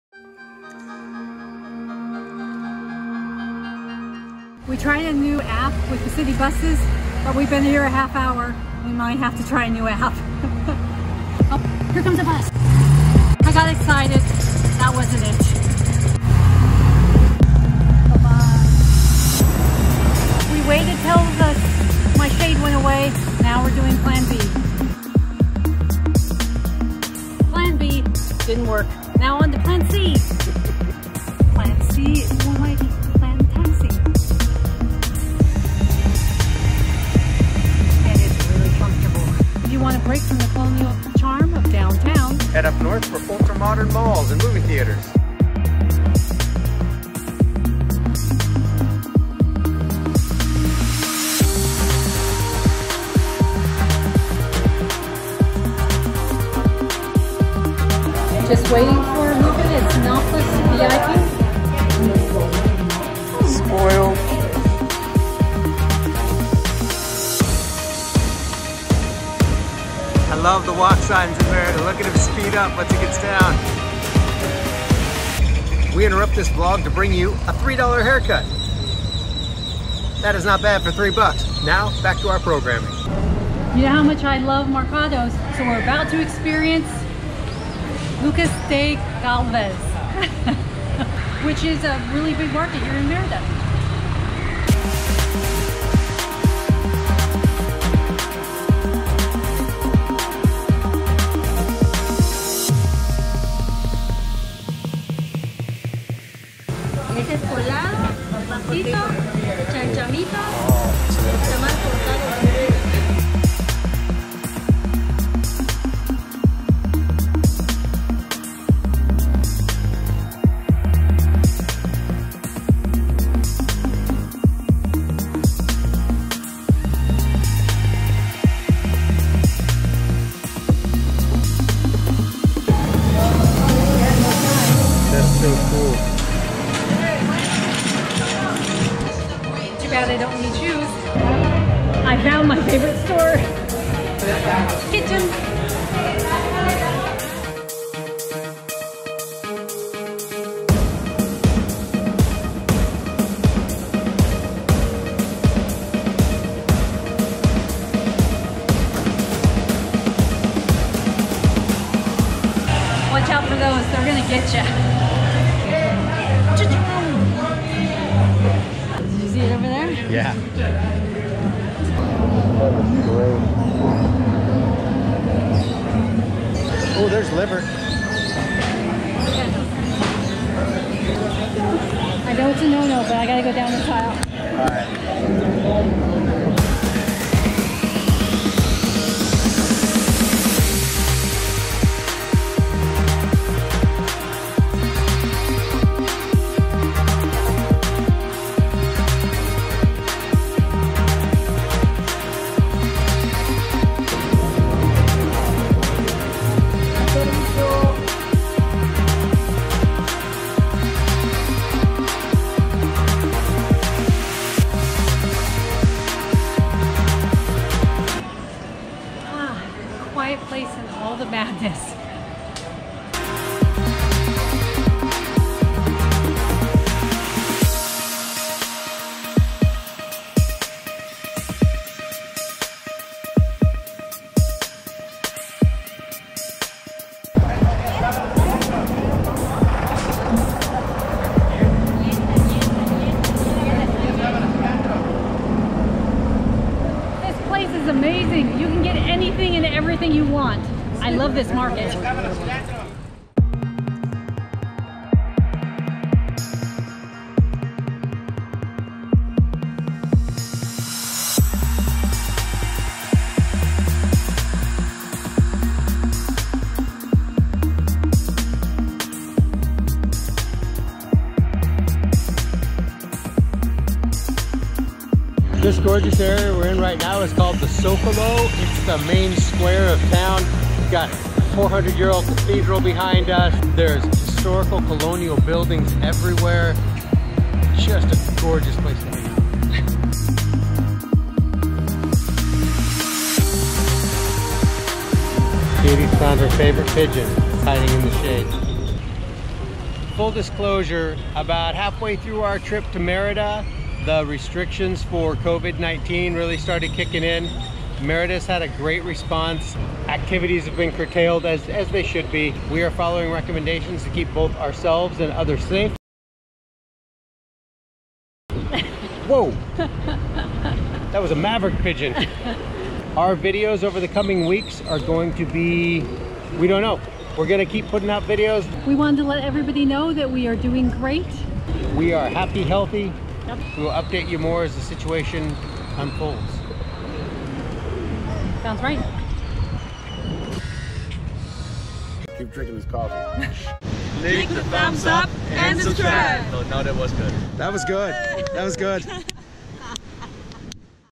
Mexico \\ MERIDA visit cut short by COVID-19 // Vlog#151
While exploring all that Merida has to offer, including a visit to the Harbor Mall and Cinepolis VIP movie theater north of Centro Historico,  COVID19 restrictions began and it became necessary for us to depart.  Thankfully we got to tour the amazing Galvis Mercado first.

Cameras:
GoPro Hero 7 Black
iPhone 11
Editing: Adobe Premier Elements 14